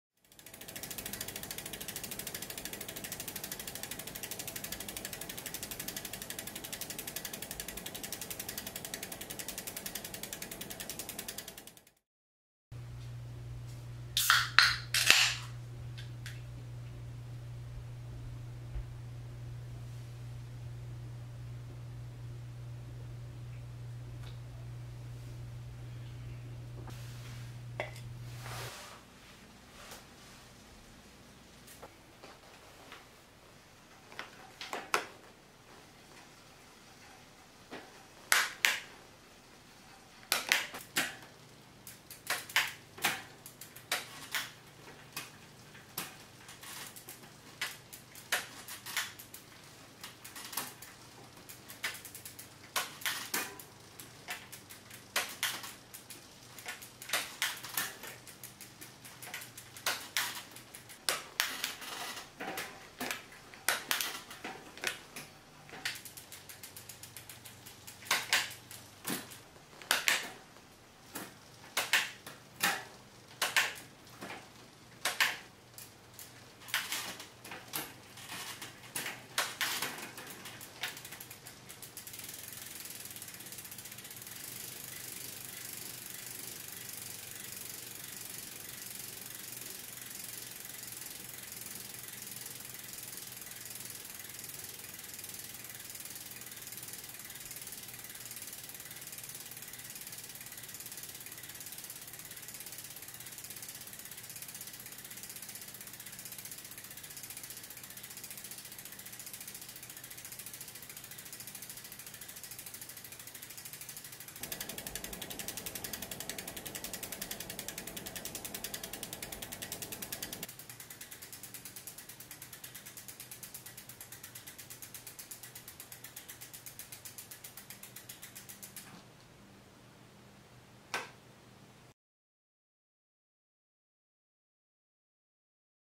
Spinning Wheel
Drinking beer in the garage. Bike on a work stand. Spinning the rear hub and clicking through gears while cats watch.